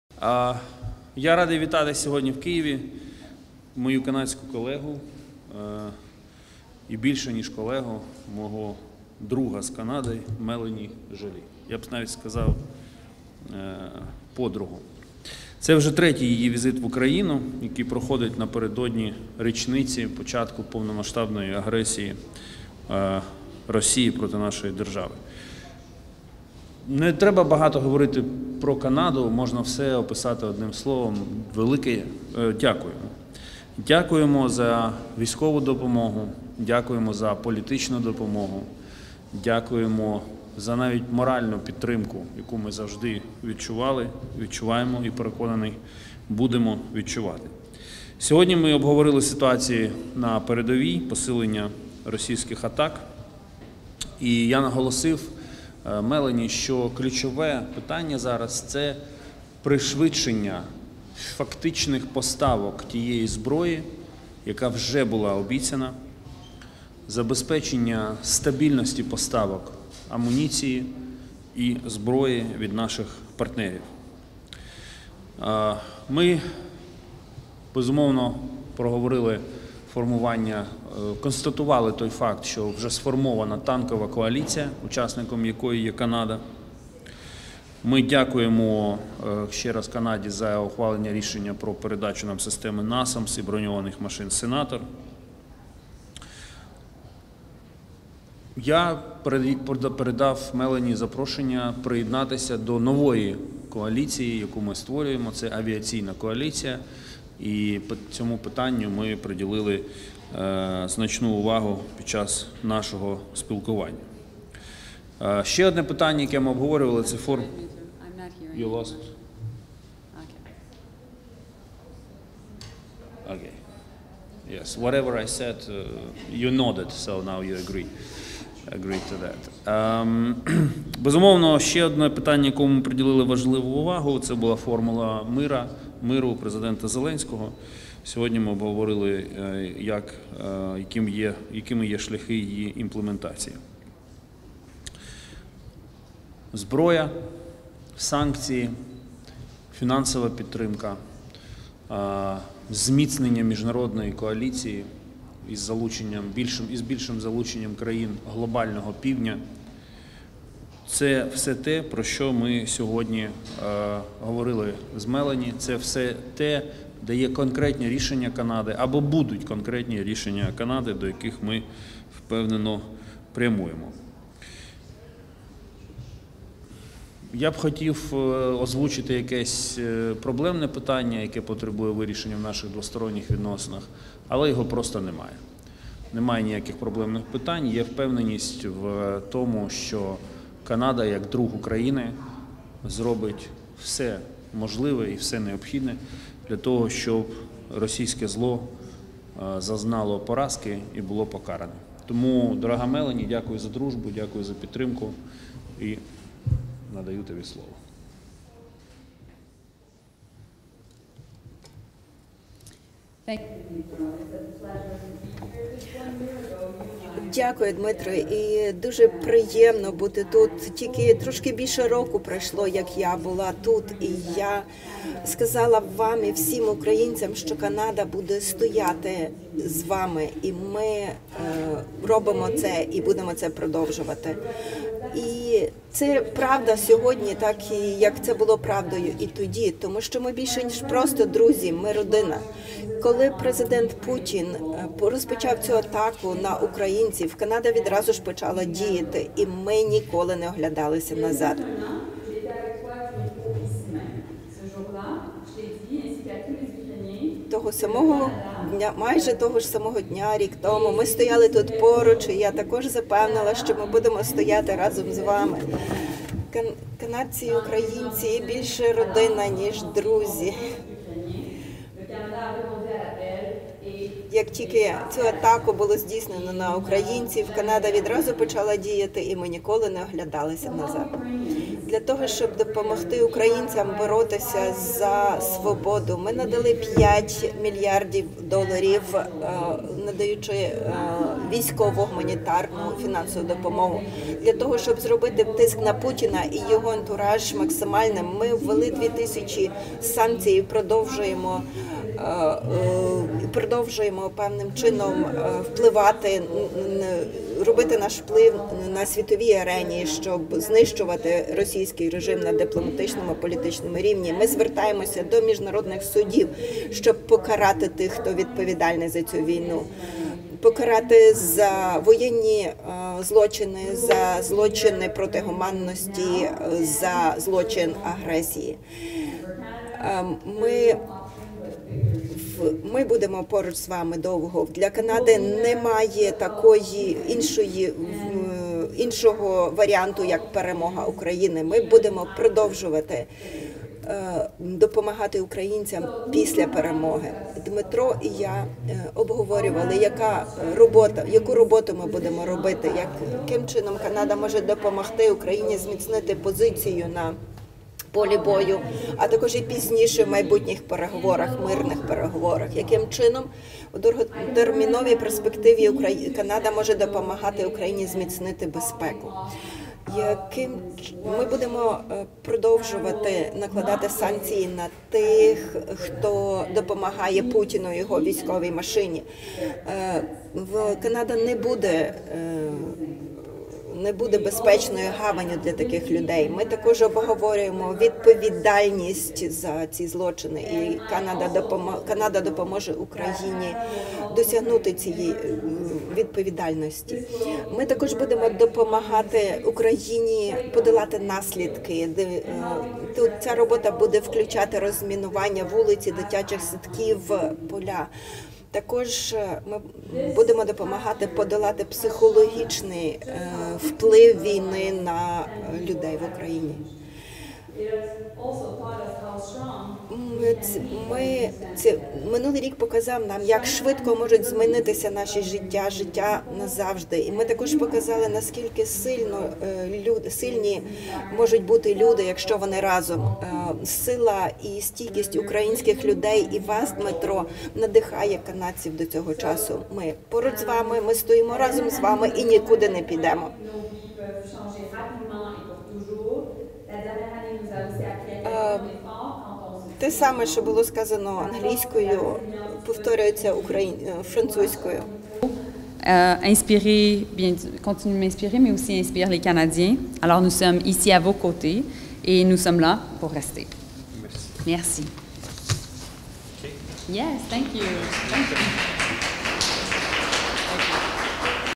Спільний БРИФІНГ очільників українського та канадського МЗС
Міністр закордонних справ Дмитро Кулеба зустрівся в Києві з канадською колегою Мелані Жолі. Вони провели спільний брифінг.